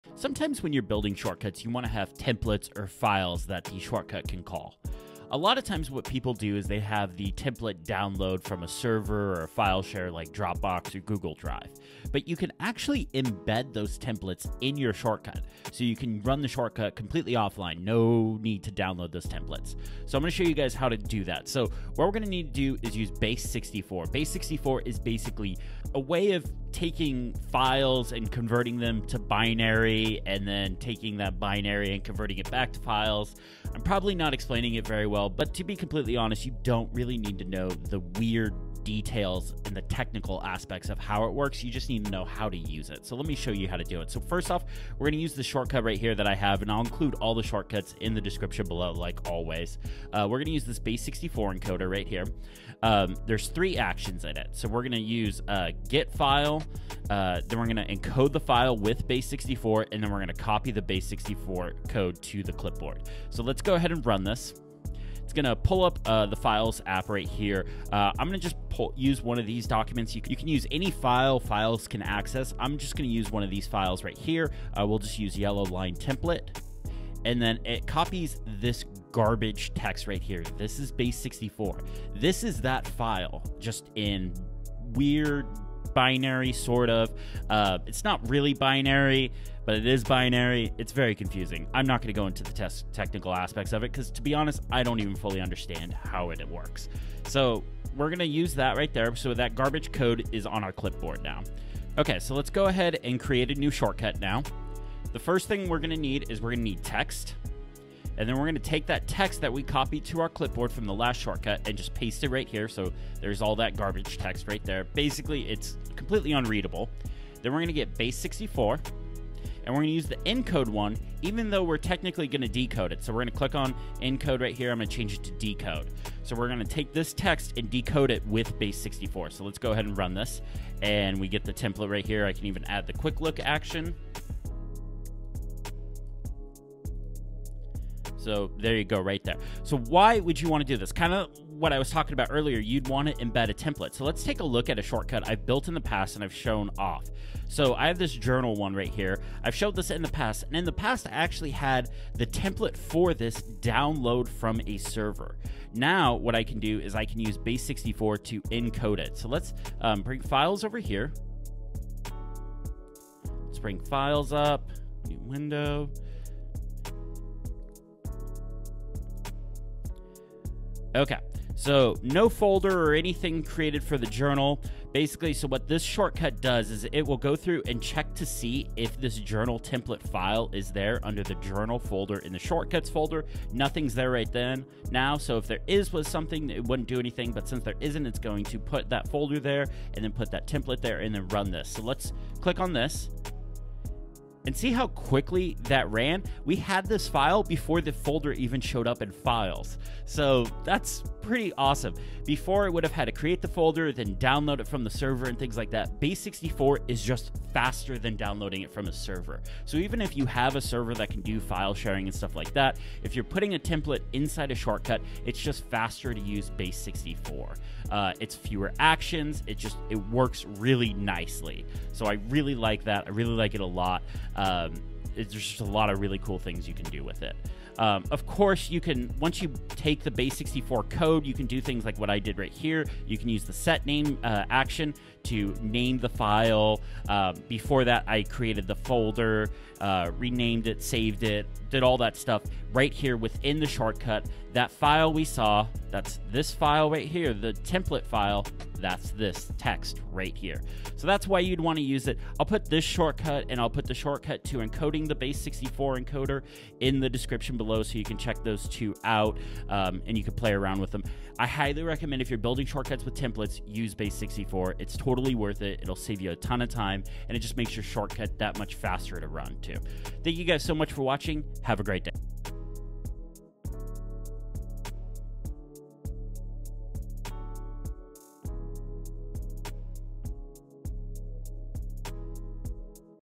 Embedding Files into Shortcuts with Base64
Let’s talk about embedding files into a shortcut with Base64.